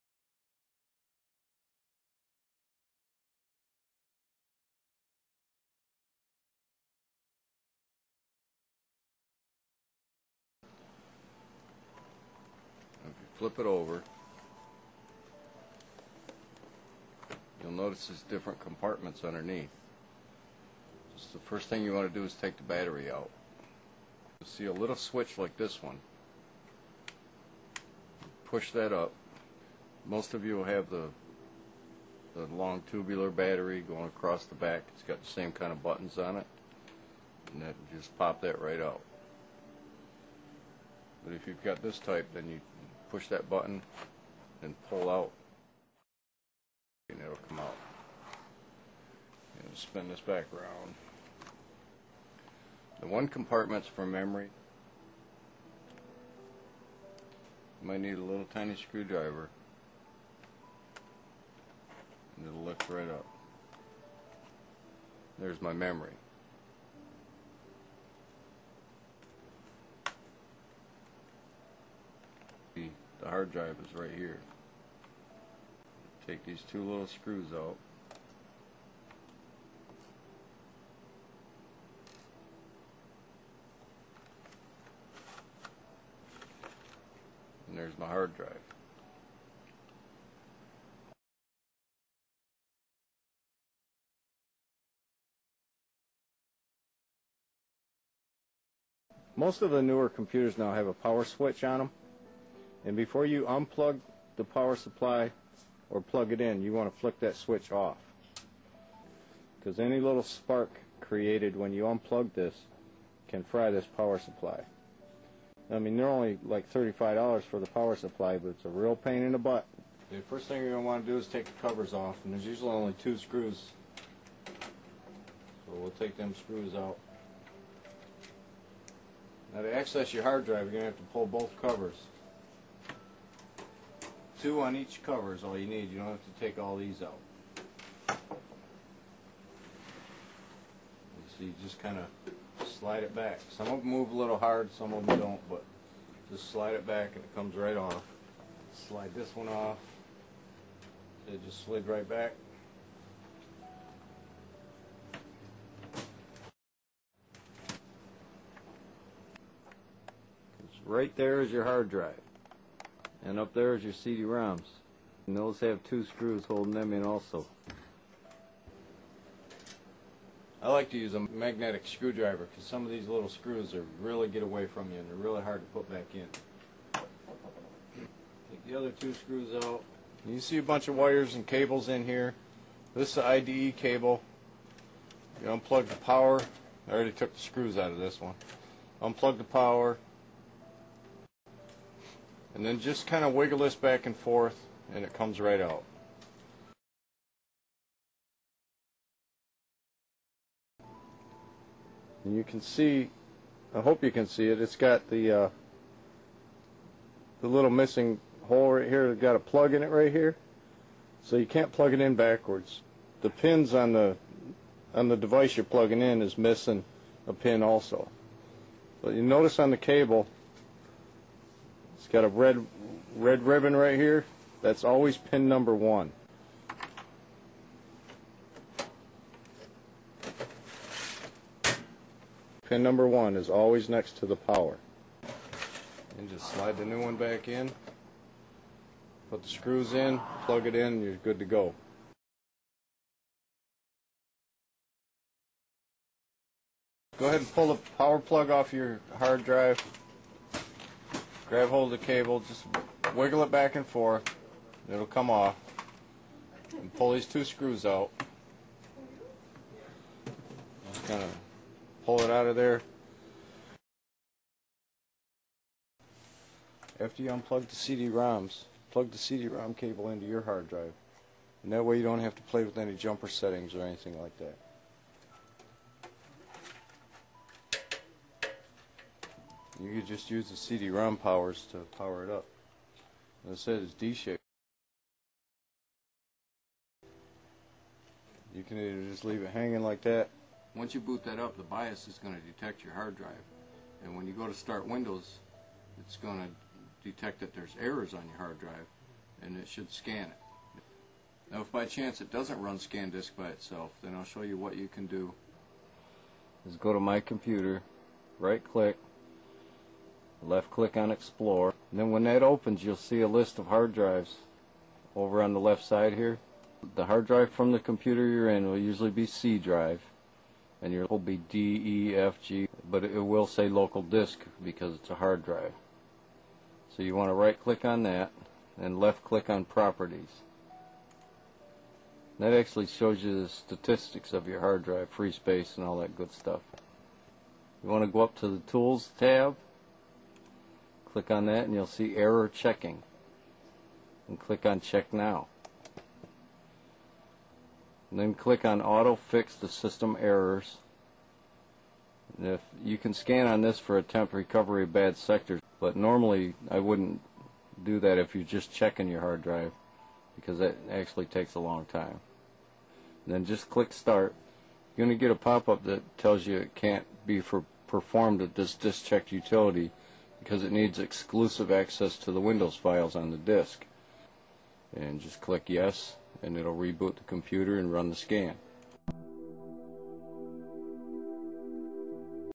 How-To Replace Your Hard Drive (updated)
This is a remix of the original to enhance the delivery of the tutorial content.
This video explains how to run an error check on your hard drive and install or replace a cd or dvd rom, and remove or replace your drive from a lap top and a desktop pc.

EDIT EDIT EDIT